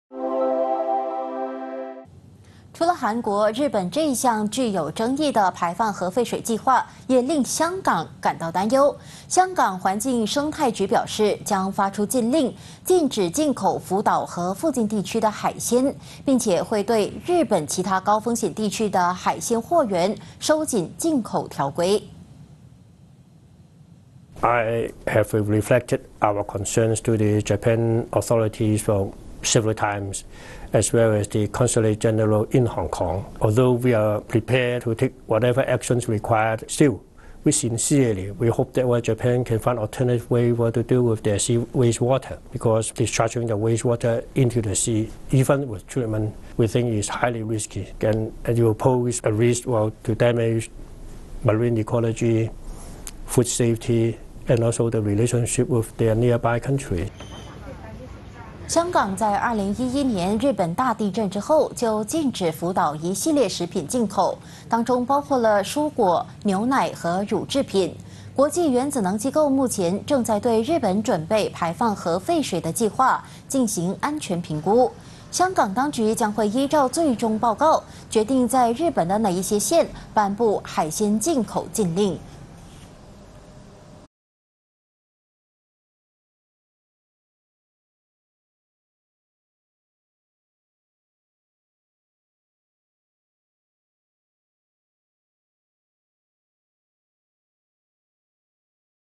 福岛核污水排海威胁食安 香港或扩大日本水产禁令 【2023.06.29 八度空间华语新闻】
【福岛核污水排海威胁食安  香港或扩大日本水产禁令 】
日本这项具有争议的排放核废水计划令香港感到担忧。香港环境生态局表示将发出禁令，禁止进口福岛和附近地区的海鲜，并会对日本其他高风险地区的海鲜货源收紧进口条规。

香港在2011年日本大地震之后，就禁止福岛一系列食品进口。当中包括了蔬果、牛奶和乳制品。国际原子能机构目前正对日本准备排放核废水的计划进行安全评估。香港当局将会依照最终报告决定在日本哪些县颁布海鲜进口禁令。

【今日主播】陈嘉荣 / 周秀洋